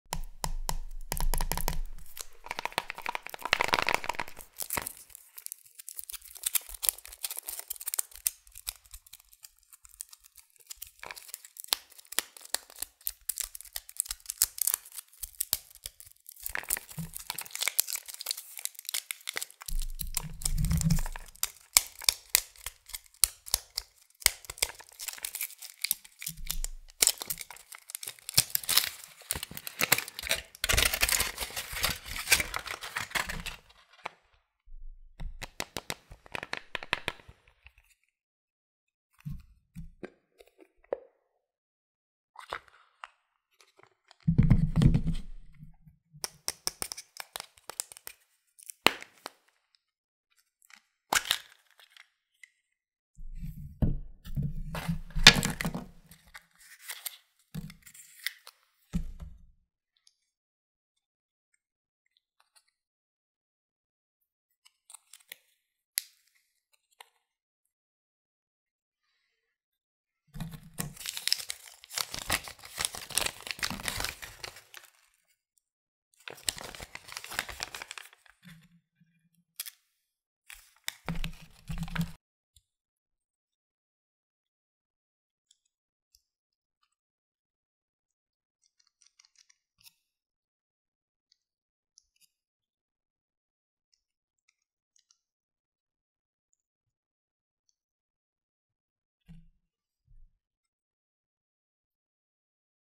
Easter 2024 ASMR: Opening Kinder Surprise Pink Bunny Egg | Relaxing Unboxing Experience!
Join us for a soothing ASMR journey this Easter 2024 as we unwrap a delightful Kinder Surprise Egg shaped like a bunny! Experience the gentle crinkles as we reveal the hidden surprises within. From adorable toys to delicious chocolate treats, each unwrapping brings a sense of joy and relaxation. Whether you're celebrating Easter or simply seeking a calming sensory experience, this video is sure to evoke tingles and smiles. Sit back, relax, and let the tranquil sounds of ASMR guide you through this enchanting unboxing adventure!